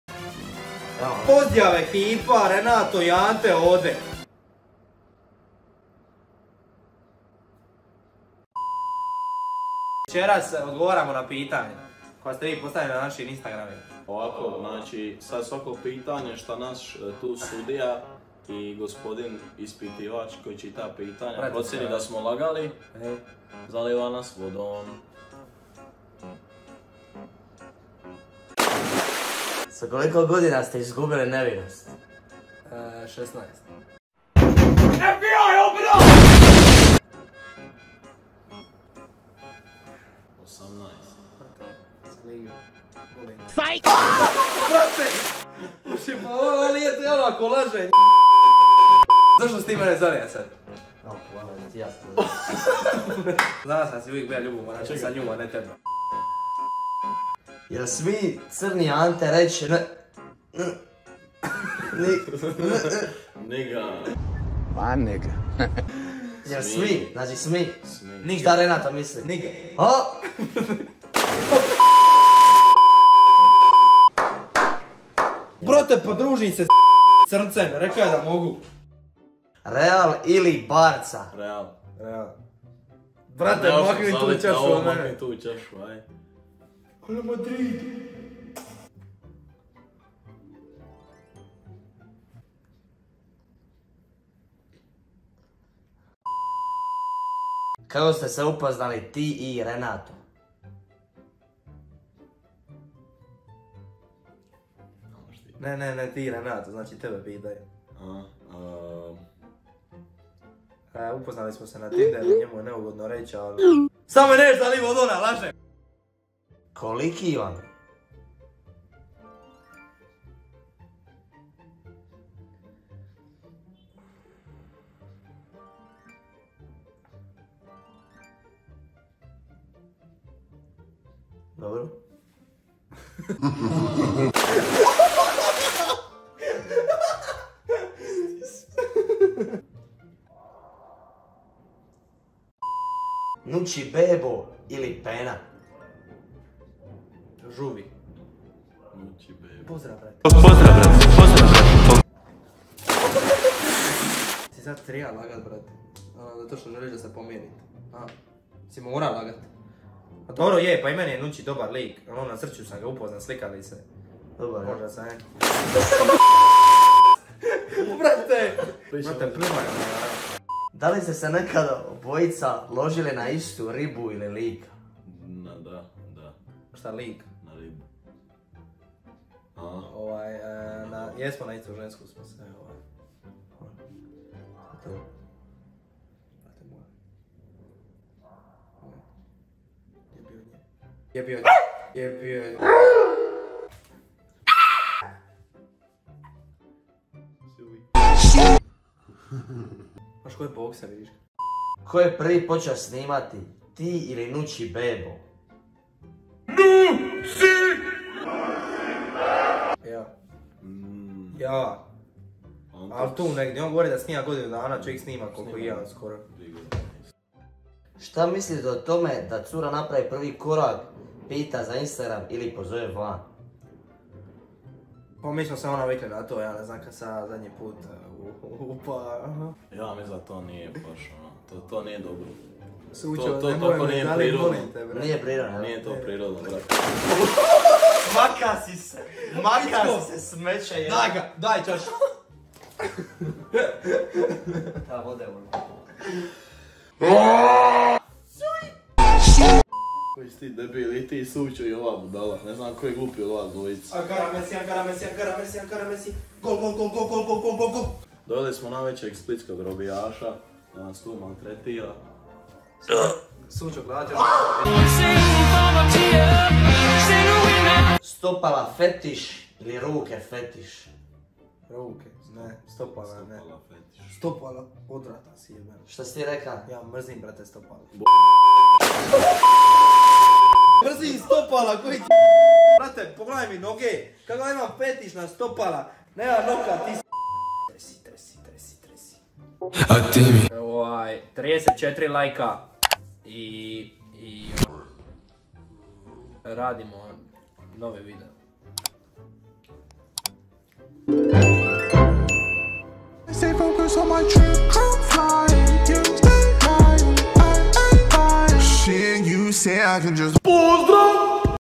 NAŠ PRVI VIDEO | Q&A *Deni ili Ante?*
Koliko zarađujemo, koliki nam je i tko nam je najdraži tiktoker? Sve možete saznati u videu!